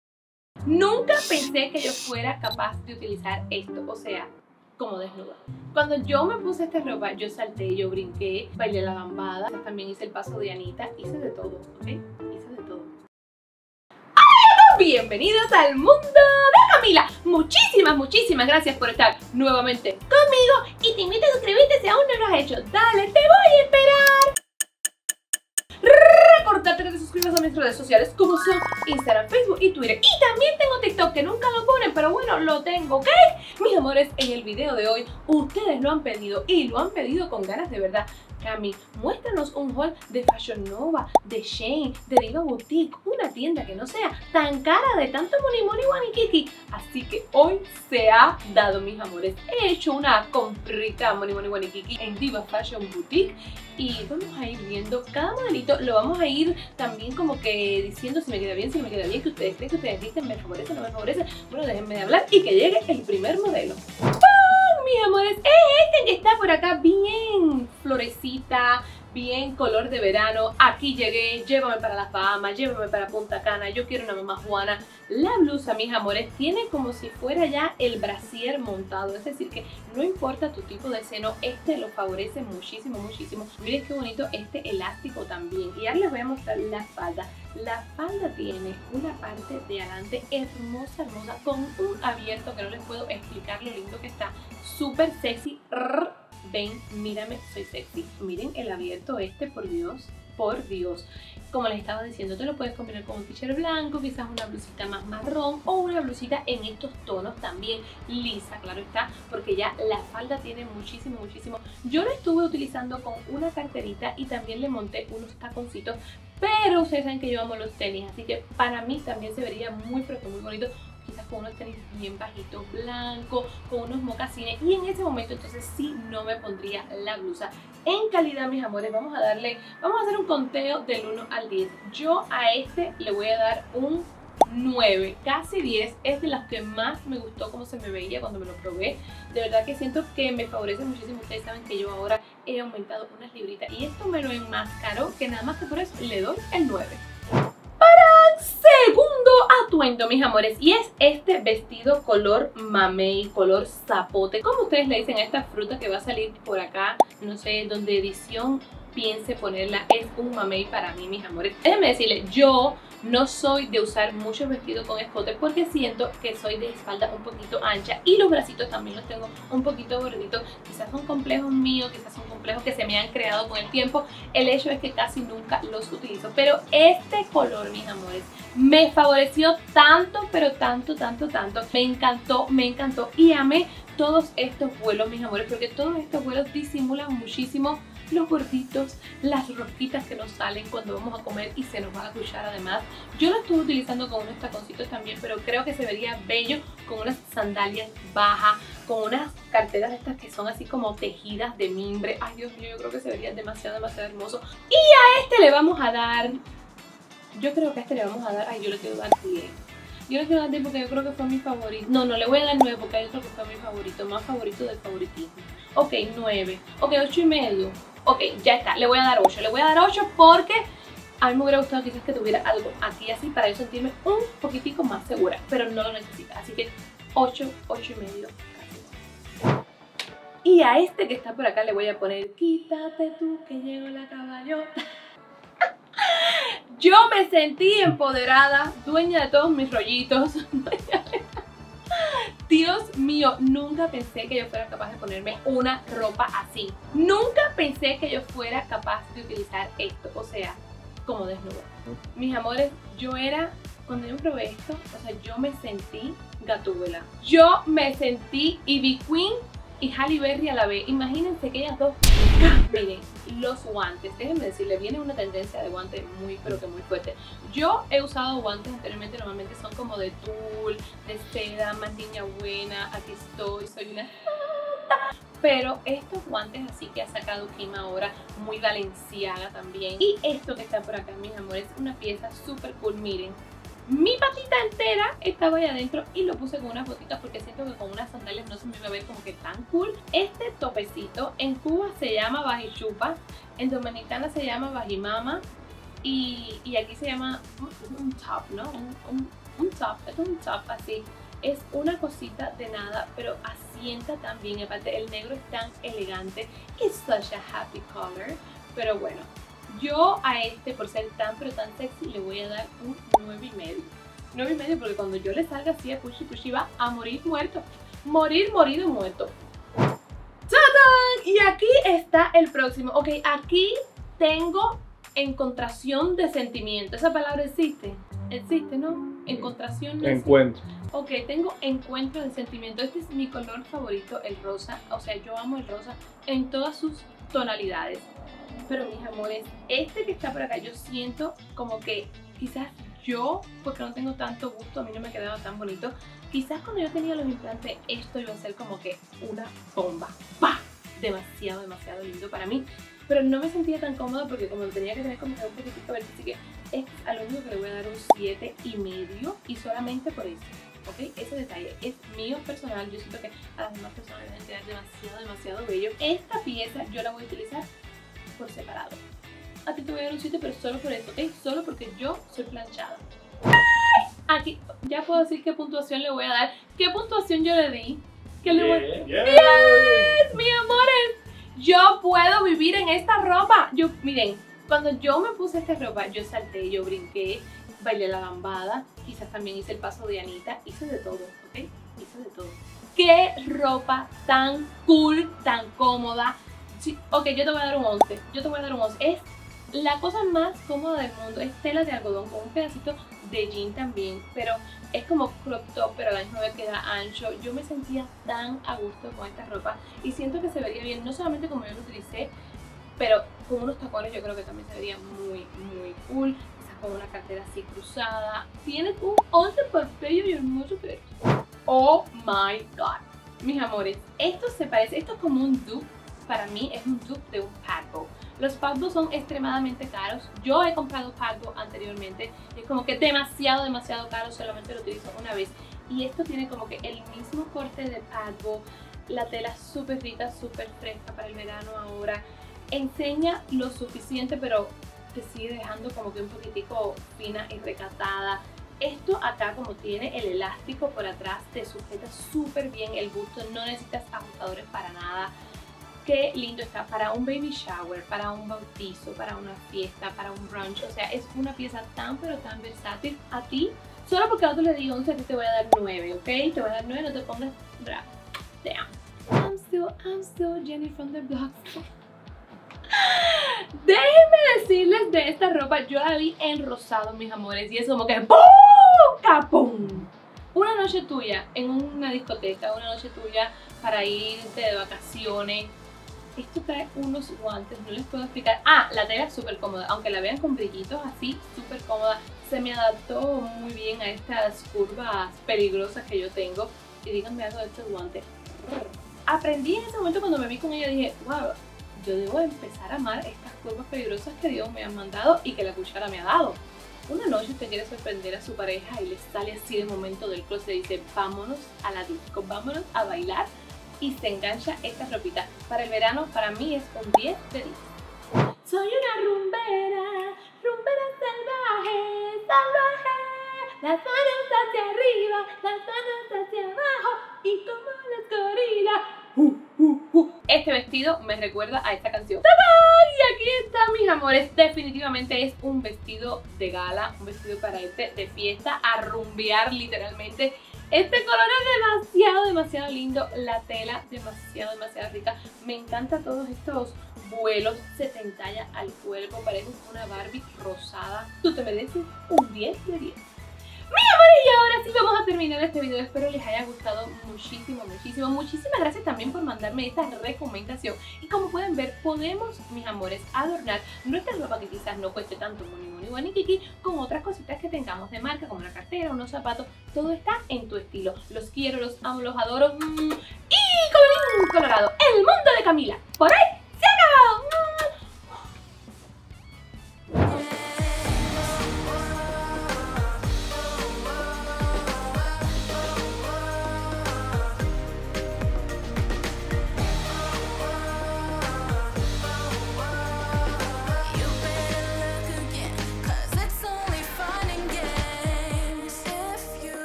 SHEIN O FASHION NOVA? 😅 | Haul de Ropa Económica 🔥 | El Mundo de Camila
Les traigo el Haul de Ropa Económica que tanto pero tanto me han pedido, deja tu comentario de la que más te ha gustado y dale mucho, mucho amor al video para una segunda parte.

LOS QUIERO LOS AMO LOS ADORO!